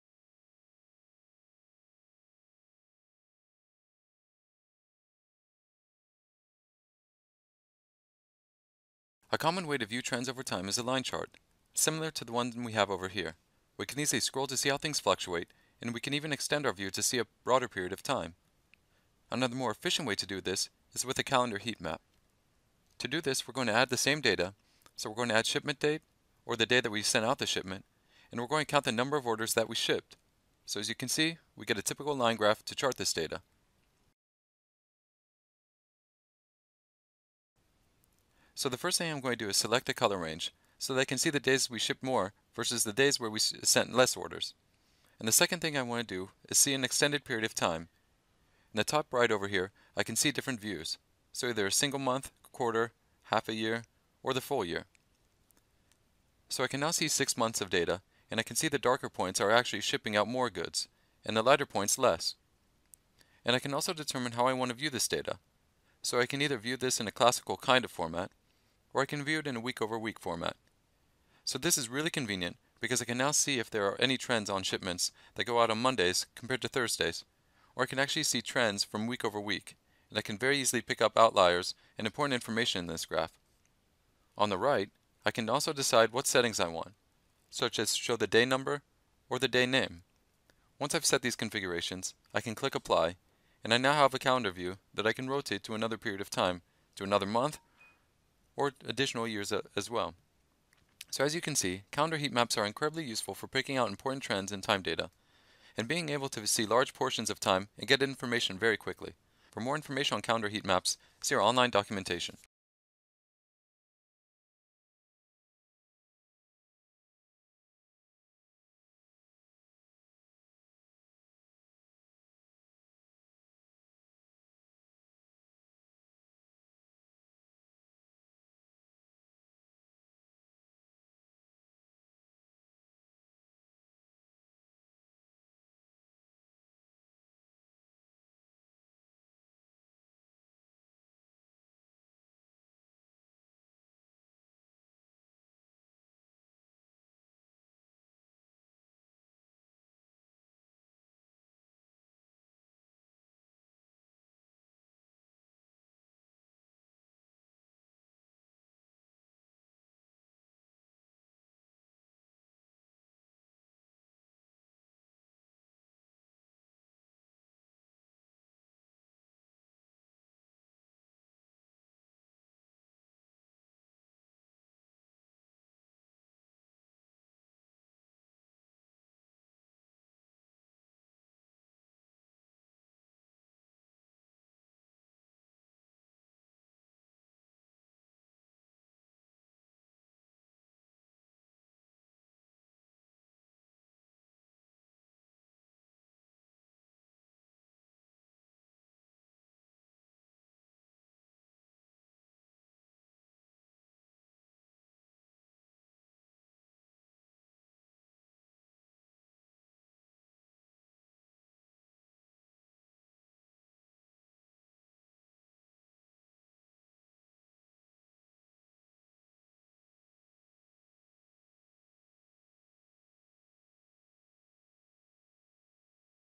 Calendar Heatmap Overview | Sisense Tutorials: Creating your Dashboard
Watch a short overview of Calendar Heatmaps in Sisense